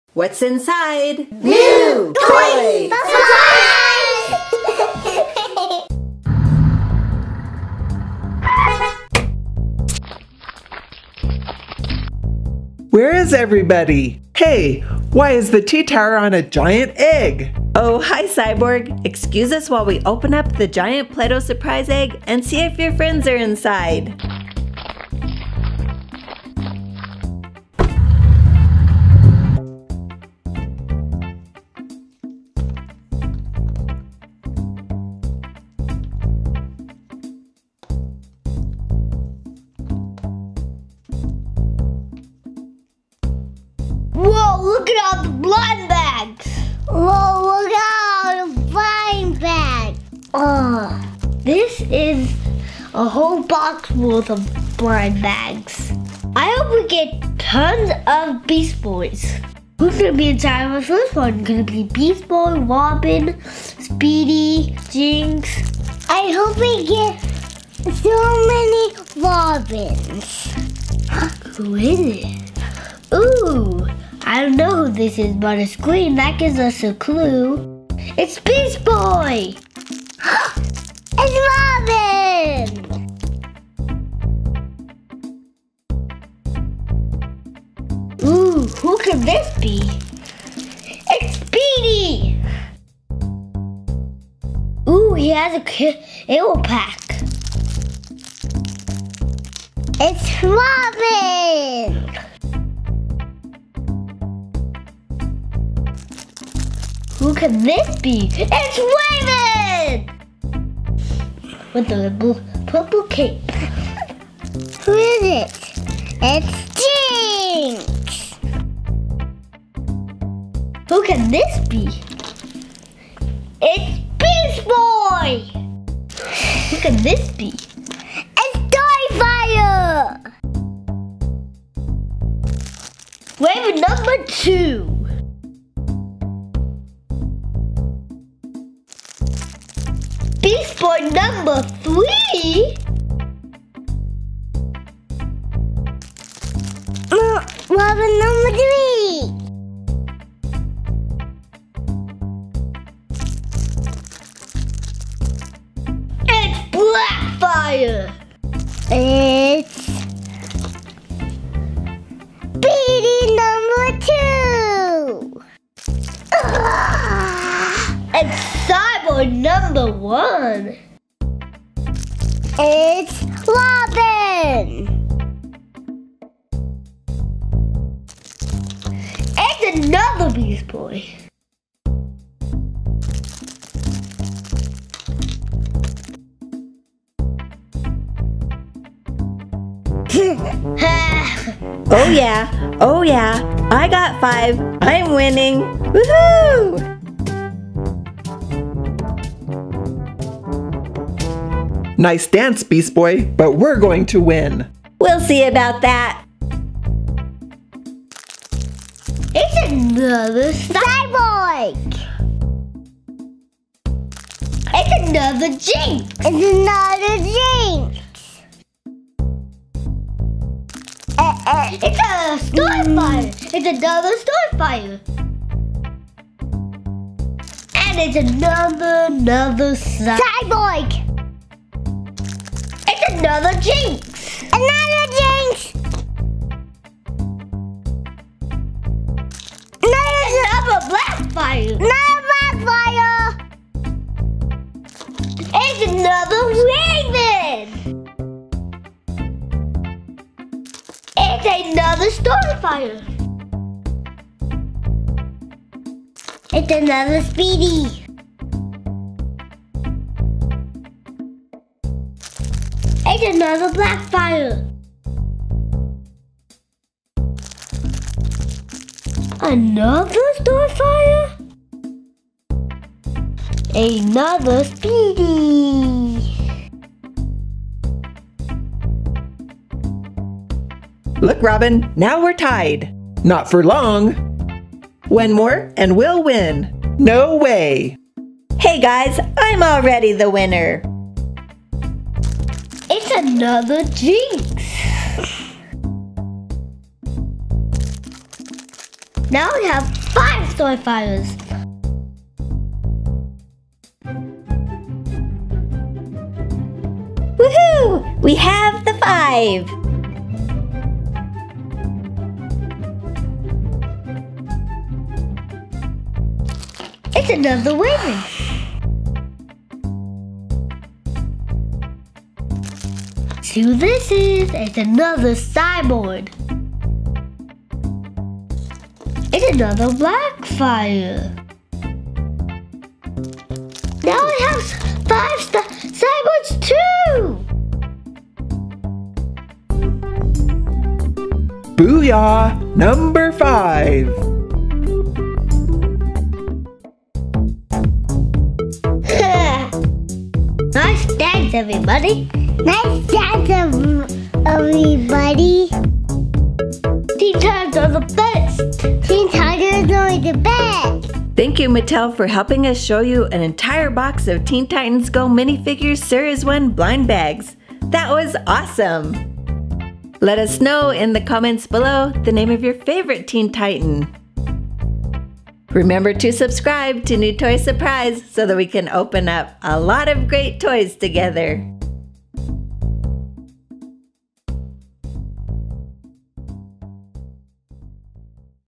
A WHOLE BOX of TEEN TITANS GO! Mini Figure Blind Bags Series 1 inside of a Play-Doh Surprise Egg!
Play-Doh Surprise Egg full of an entire box of  Teen Titans Go!  Mini Figure Blind Bags Series 1 is so fun to open!! Will Beast Boy, Robin, Cyborg, Starfire, Raven, Jinx, Speedy or Blackfire win the contest? A big Thank You to Mattel for helping us show these awesome Teen Titans!

Don't miss these other great toy surprises!